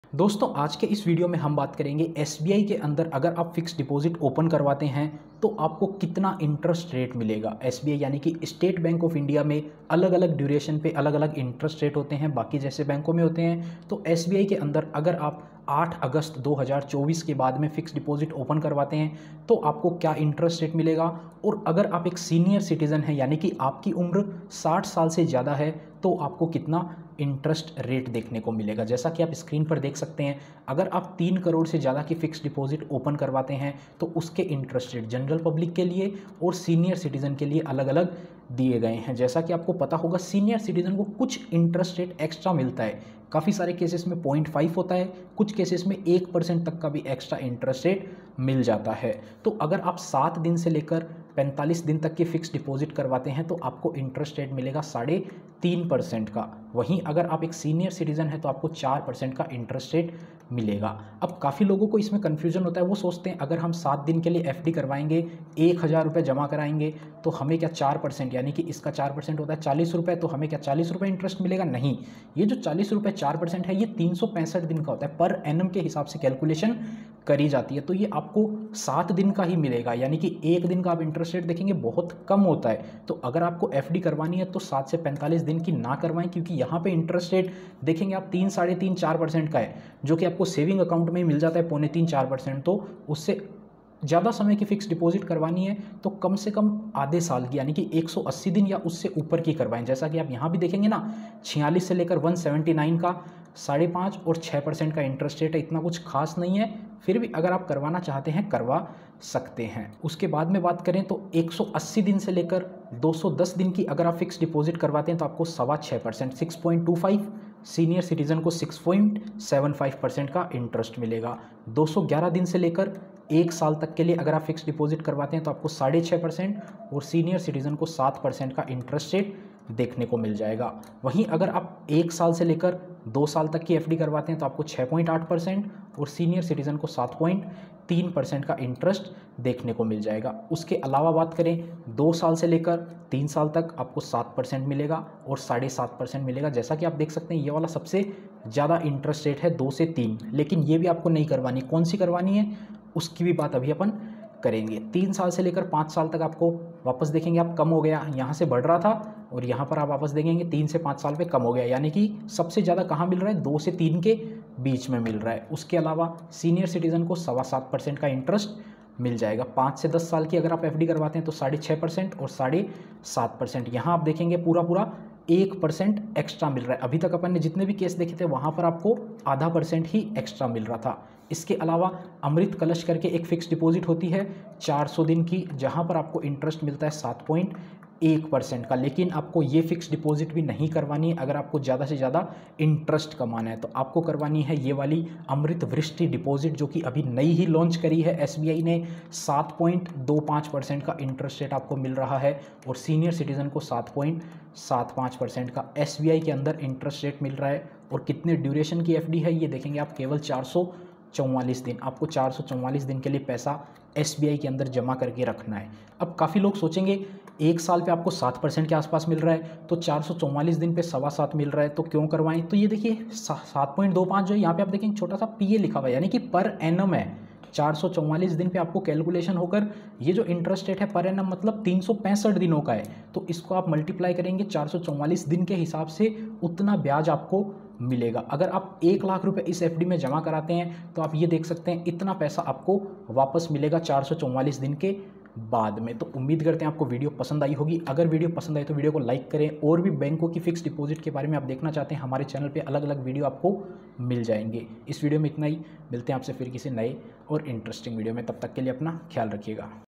SBI FD Interest Rates 2024 | SBI Fixed Deposit Interest Rates 2024 | Latest FD Rates August 2024
In this video we have discussed about the fixed deposit interest rates in State Bank of India for different tenures. and sbi increased fd rates on 08 August 2024 for different tenures. and Amrit kalash deposit tennure and Amrit Vristhi deposit is also increased.